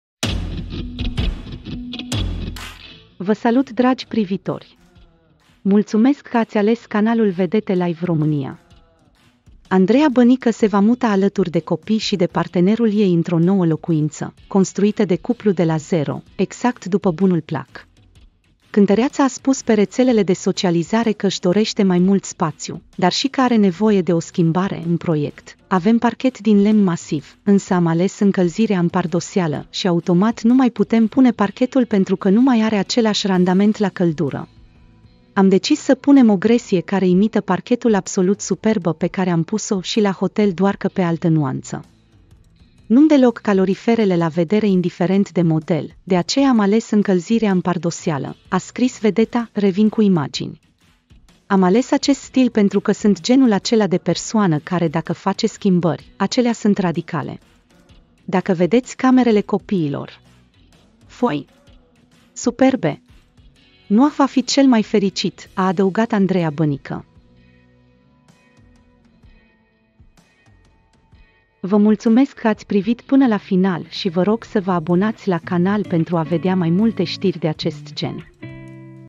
Andreea Bănică se mută în casă nouă. Imagini din vila de lux a vedetei care este în construcție
Cum arată hotelul Andreei Bănică din stațiunea Eforie Nord.  ARTISTA IL VA DESCHIDE PE 15 IULIE